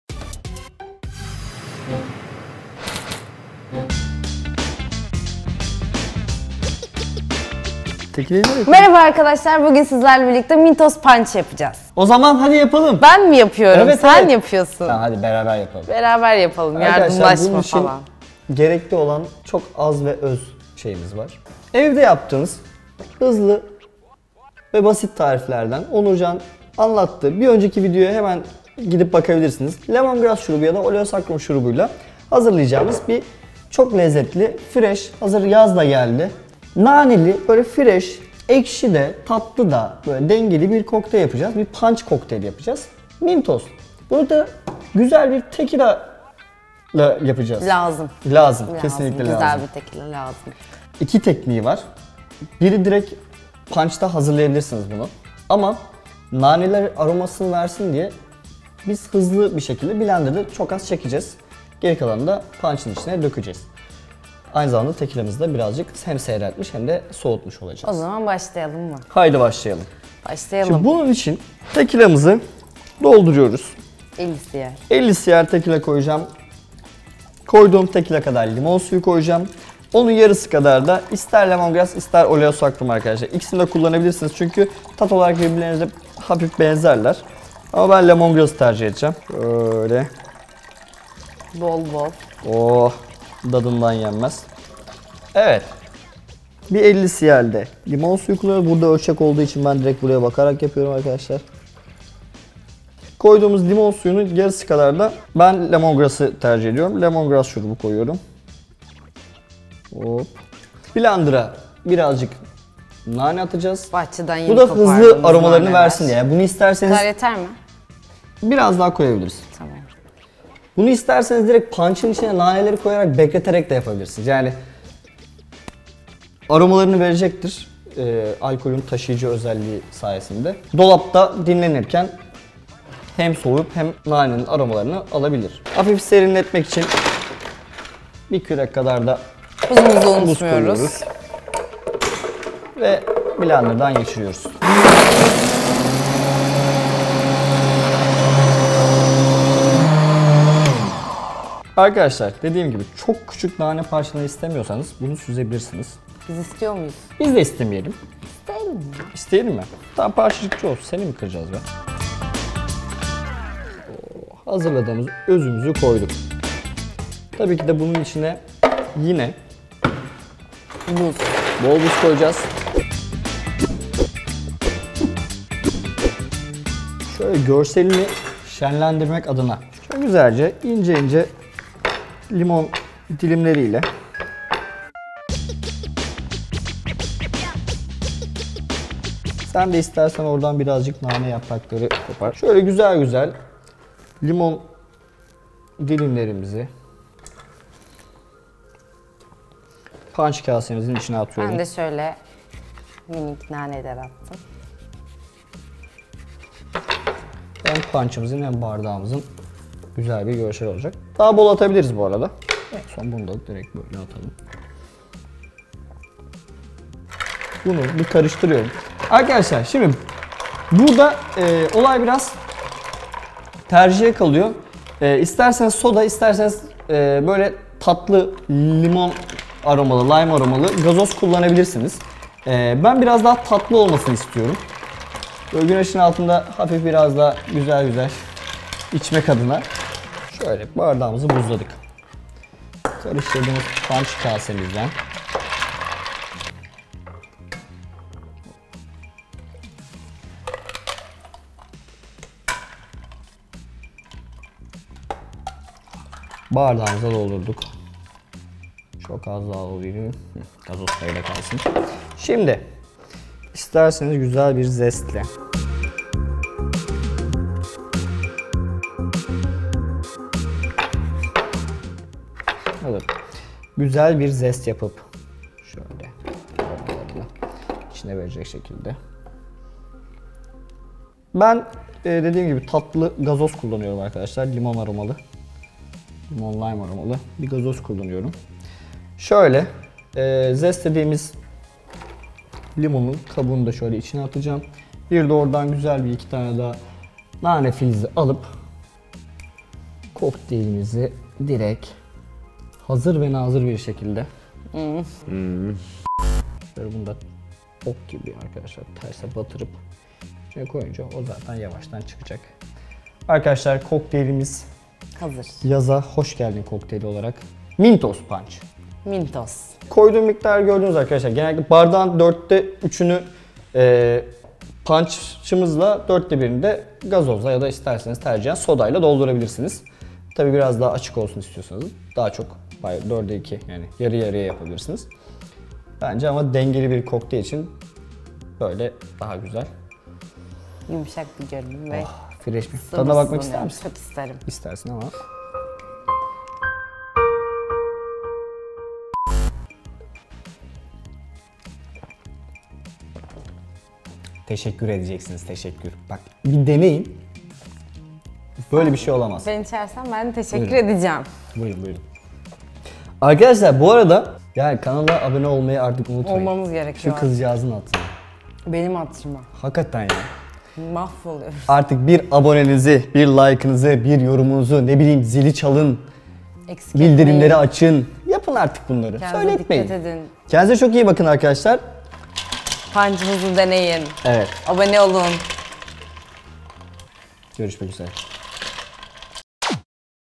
Pratik ve Ferah: Mintos Punch
PartyTubecular! Hazır havalar da ısınıyorken, kendinize naneli ve lemongrass şuruplu bir sürahi kokteyli hazırlamak ister misiniz? İşte Bircan ve Beyza'dan sizin için evinizdeki partilerde kolayca hazırlayabileceğiniz tekilalı Mintos Punch tarifi!